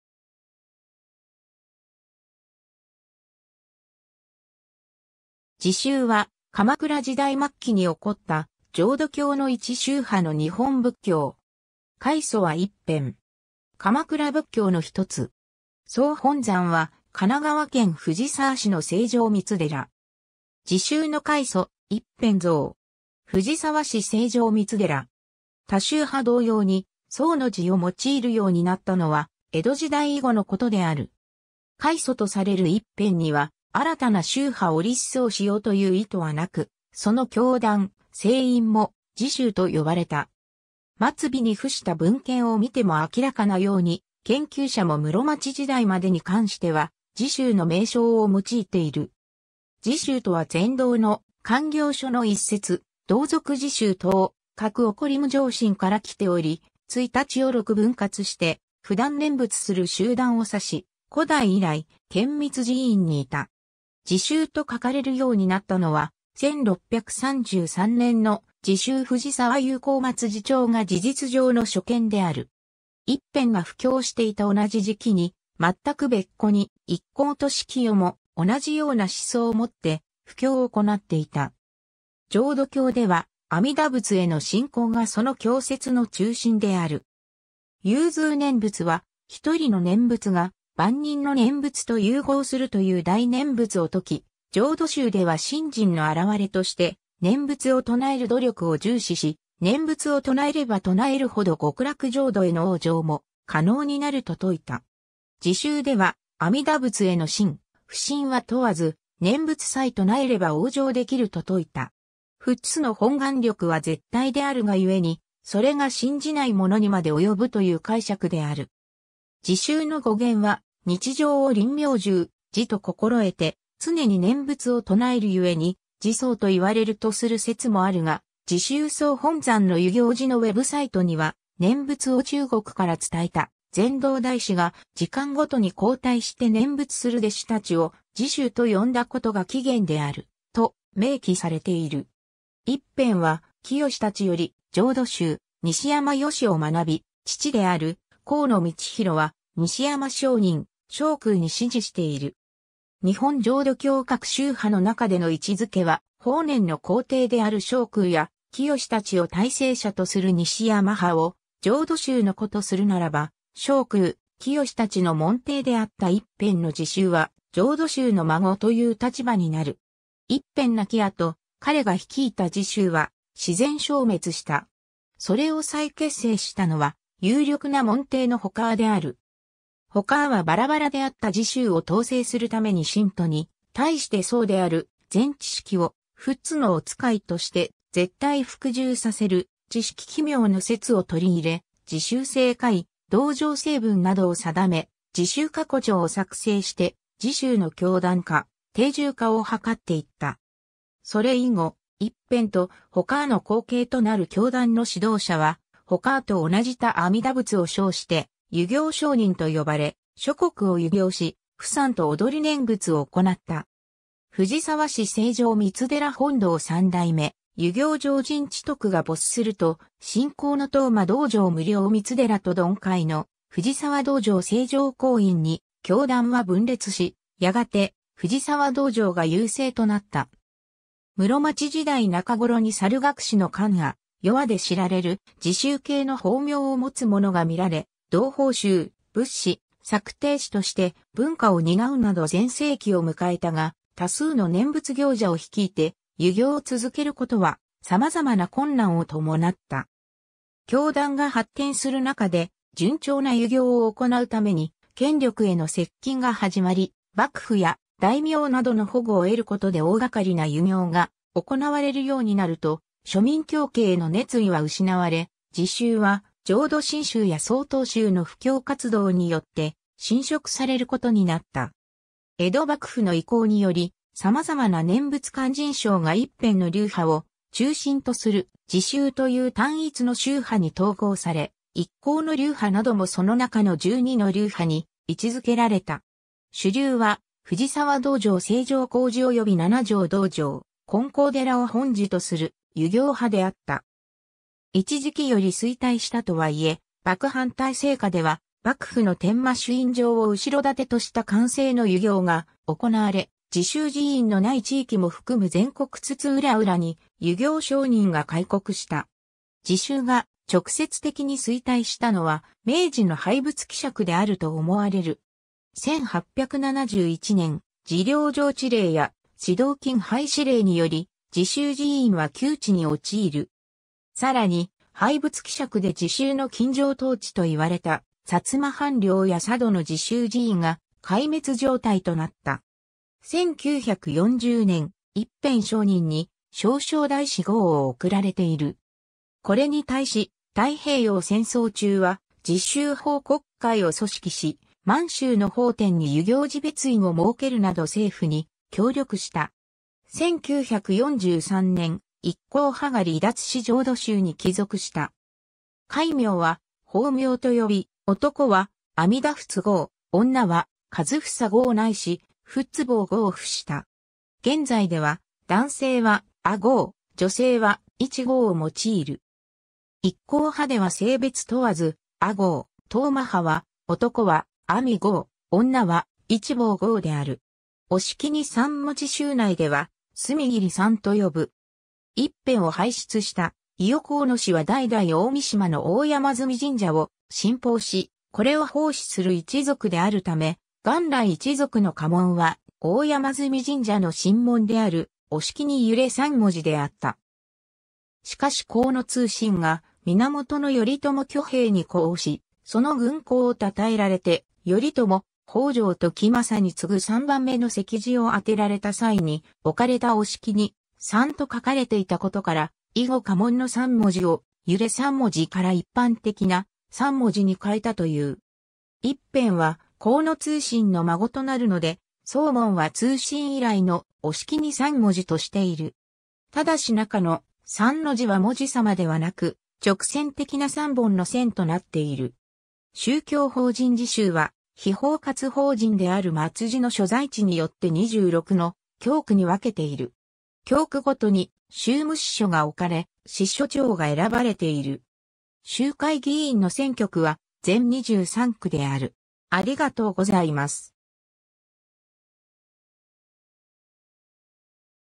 時宗
時宗（じしゅう）は、鎌倉時代末期に興った浄土教の一宗派の日本仏教。
開祖は一遍。
鎌倉仏教のひとつ。
総本山は神奈川県藤沢市の清浄光寺（通称遊行寺）。
 時宗の開祖・一遍像。
藤沢市清浄光寺。
 他宗派同様に「宗」の字を用いるようになったのは、江戸時代以後のことである。
開祖とされる一遍には新たな宗派を立宗しようという意図はなく、その教団・成員も「時衆」と呼ばれた。
末尾に附した文献を見ても明らかなように、研究者も室町時代までに関しては時衆の名称を用いている。
時衆とは善導の「観経疏」の一節「道俗時衆等、各發無上心」から来ており、一日を6分割して不断念仏する集団（ないし成員）を指し、古代以来、顕密寺院にいた。
「時宗」と書かれるようになったのは、1633年（寛永10年）の『時宗藤沢遊行末寺帳』が事実上の初見である。
一遍が布教していた同じ時期に、全く別個に一向俊聖も同じような思想を持って布教を行っていた。
 浄土教では阿弥陀仏（阿彌陀佛）への信仰がその教説の中心である。
融通念仏は、一人の念仏が万人の念仏と融合するという大念仏を説き、浄土宗では信心の表れとして念仏を唱える努力を重視し、念仏を唱えれば唱えるほど極楽浄土への往生も可能になると説いた。
 時宗では、阿弥陀仏への信・不信は問わず、念仏さえ唱えれば往生できると説いた。
仏の本願力は絶対であるがゆえに、それが信じない者にまで及ぶという解釈である。
 時宗（時衆）の語源は、「日常を臨命終「時」（臨終）と心得て、常に念仏を唱える故に「時」宗といわれる」とする説もあるが、時宗総本山の遊行寺のウェブサイトには念仏を中国から伝えた善導大師が時間ごとに交代して念仏する弟子たちを「時衆」と呼んだ事が起源である、と明記されている。
 一遍は聖達より浄土宗西山義を学び、父である河野通広（如仏）は西山上人・証空に師事している。
日本浄土教各宗派の中での位置付けは、法然の高弟である証空や聖達を大成者とする西山派を｢浄土宗の子｣とするならば、証空・聖達の門弟であった一遍の時宗は｢浄土宗の孫｣という立場になる。
 一遍亡き後、彼が率いた時衆は自然消滅した。
それを再結成したのは、有力な門弟の他阿（他阿弥陀仏）である。
他阿はバラバラであった時衆を統制するために信徒に対して僧である善知識を「仏の御使い」として絶対服従させる知識帰命の説を取り入れ、「時衆制誡」「道場制文」などを定め、『時衆過去帳』を作成して時衆の教団化、定住化を図っていった。
それ以後、一遍と他阿の後継となる教団の指導者は他阿と同じ他阿弥陀仏を称して遊行上人と呼ばれ、諸国を遊行し、賦算（ふさん）と踊念仏を行った。
 藤沢市清浄光寺本堂 三代目遊行上人智得が没すると、真光の当麻道場無量光寺と呑海の藤沢道場清浄光院（のちの清浄光寺）に教団は分裂し、やがて藤沢道場が優勢となった。
室町時代中頃に猿楽師の観阿（観阿弥）、世阿（世阿弥）で知られる時衆系の法名を持つ者が見られ、同朋衆、仏師、作庭師として文化を担うなど全盛期を迎えたが、多数の念仏行者を率いて遊行を続けることは、様々な困難を伴った。
教団が発展する中で、順調な遊行を行うために権力への接近が始まり、幕府や大名などの保護を得ることで大がかりな遊行が行われるようになると、庶民教化への熱意は失われ、時宗は浄土真宗や曹洞宗の布教活動によって侵食されることになった。
 江戸幕府の意向により、様々な念仏勧進聖が一遍の流派を中心とする「時宗」という単一の宗派に統合され、一向の流派などもその中の12の流派に位置付けられた（「時宗十二派」）。
主流は藤沢道場清浄光寺および七条道場金光寺を本寺とする「遊行派」であった。
一時期より衰退したとはいえ、幕藩体制下では、幕府の伝馬朱印状を後ろ盾とした官製の遊行が行われ、時宗寺院のない地域も含む全国津々浦々に、遊行上人が回国した。
時宗が直接的に衰退したのは、明治の廃仏毀釈であると思われる。
 1871年（明治4年）、寺領上知令や祠堂金廃止令により、時宗寺院は窮地に陥る。
さらに廃仏毀釈で時宗の金城湯池といわれた薩摩藩領や佐渡の時宗寺院が壊滅状態となった。
1940年（昭和15年）、一遍上人に「証誠大師」号を贈られている。
これに対し、太平洋戦争中は時宗報国会を組織し、満州の奉天に遊行寺別院を設けるなど政府に協力した。
1943年（昭和18年）、一向派が離脱し浄土宗に帰属した。
 戒名は法名と呼び、男は「阿弥陀仏」号、女は「一房」号ないし「仏房」号を附した。...